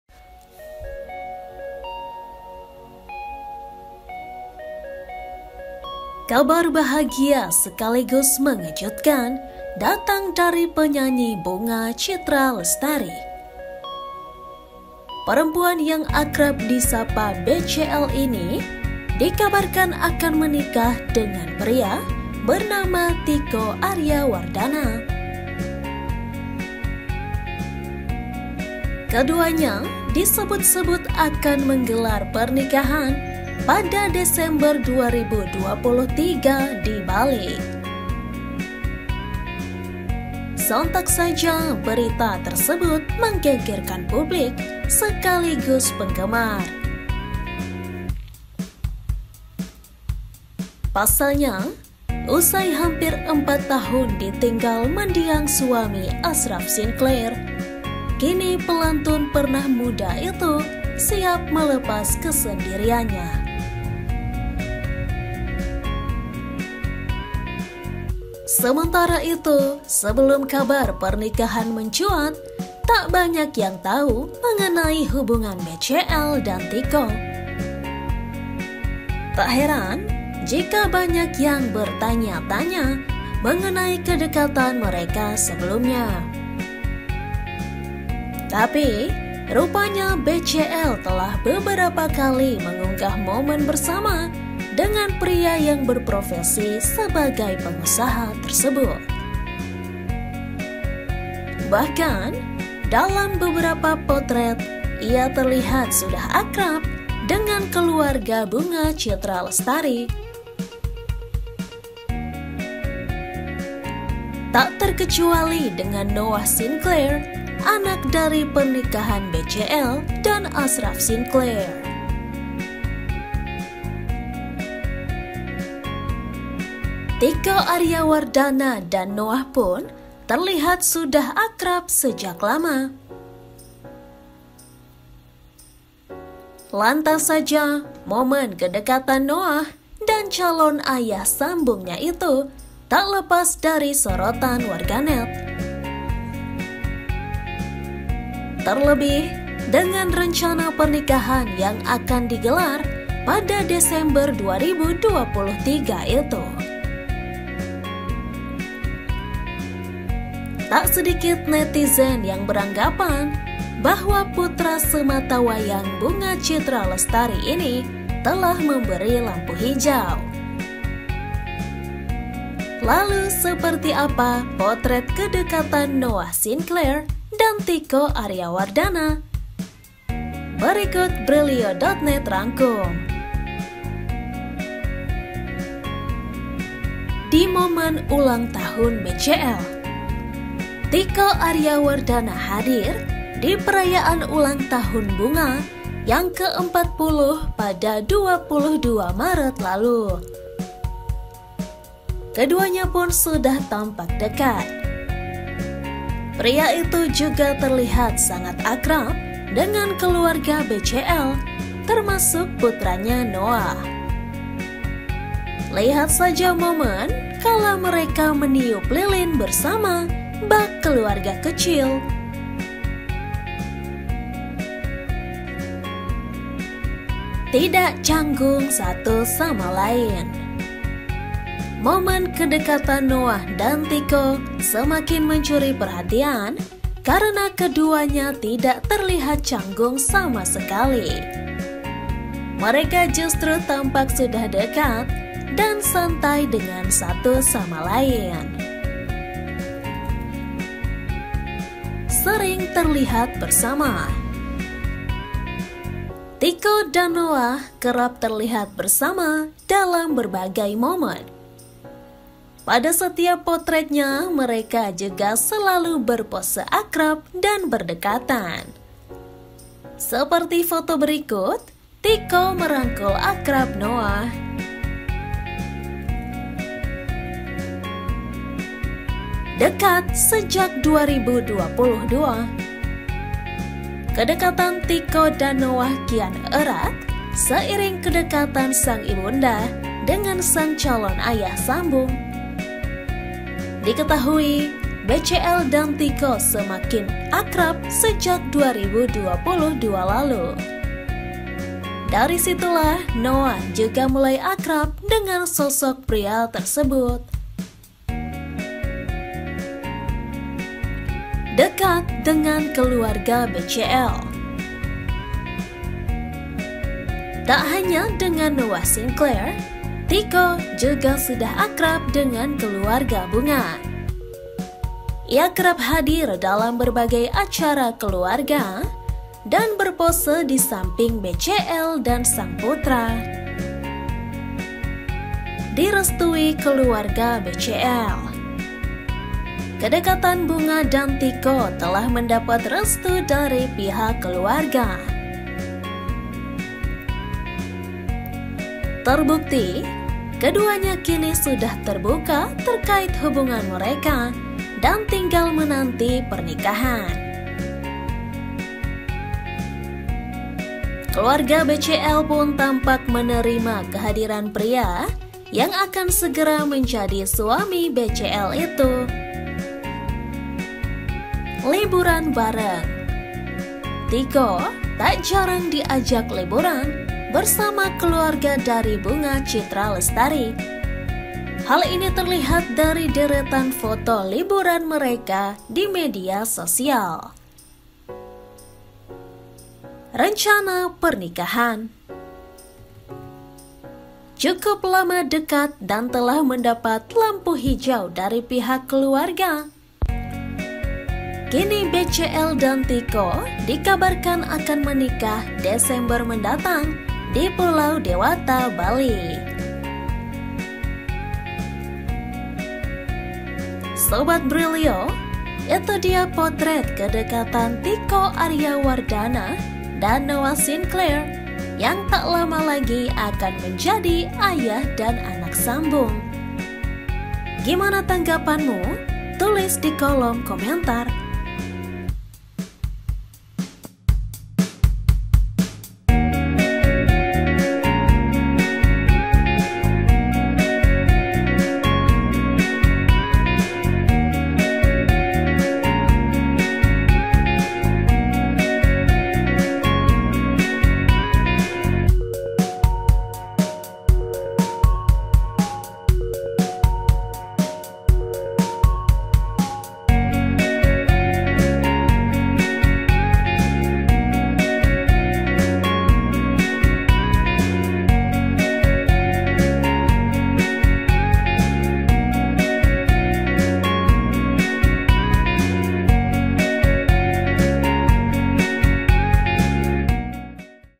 Kedekatan Noah anak BCL dengan Tiko Aryawardhana yang sebentar lagi jadi ayah sambung
Penanggung Jawab: Titis Widyatmoko
Koordinator: Agib Tanjung
Kreatif: Rahmani Dian Nuswantari | Desinta Ramadani | Deta Jauda
Editor Grafis: Restu Anggoro | Dimas Satya
Camera Person: Istrini Puji Hastuti